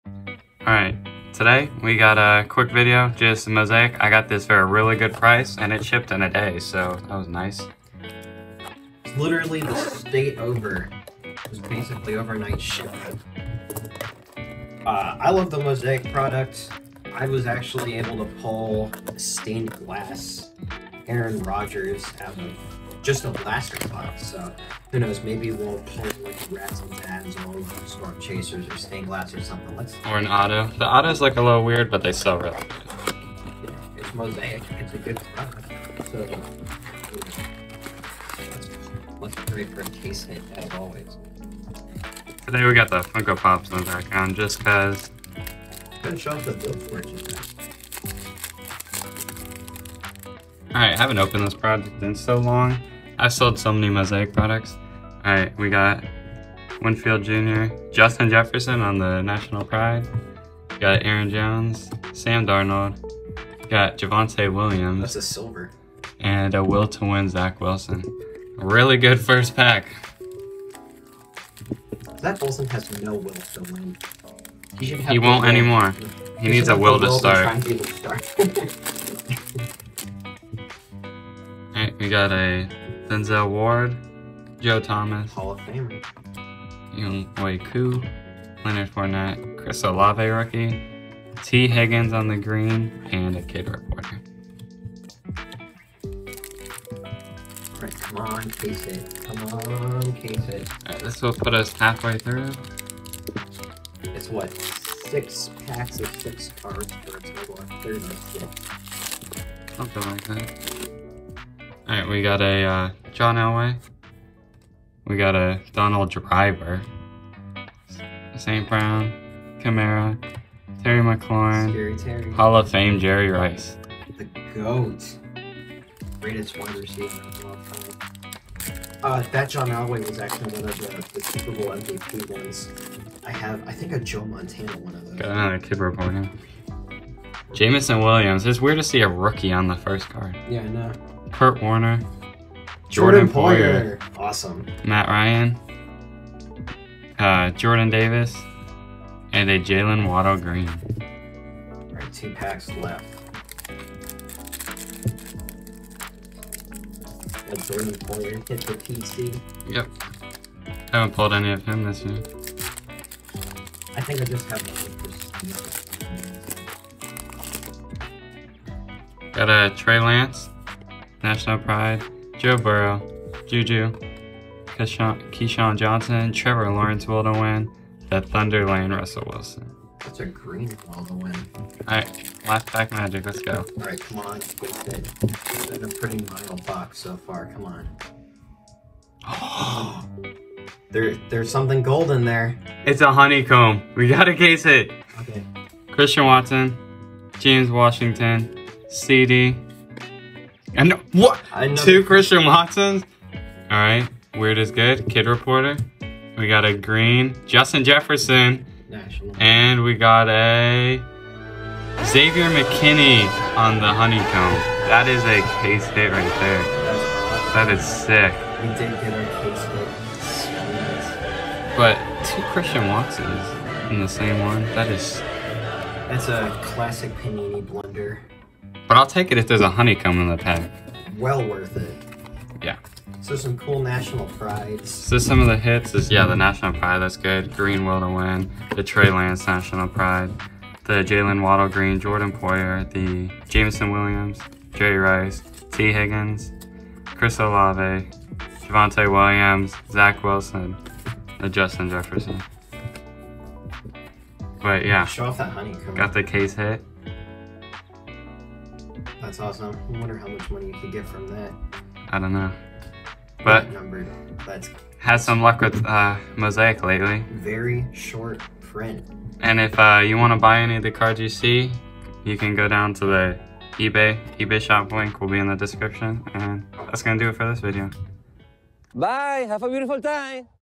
WE PULLED A HONEYCOMB! | 2022 Mosaic Blaster Unboxing
Gunnar opens up a 2022 Panini Mosaic blaster box and pulls a VERY short print HONEYCOMB parallel! Watch the video to see who it is! Always be sure to check out @gammillcards on Twitter and TikTok and check out our eBay shops down below.
Cole's eBay
Gunnar's eBay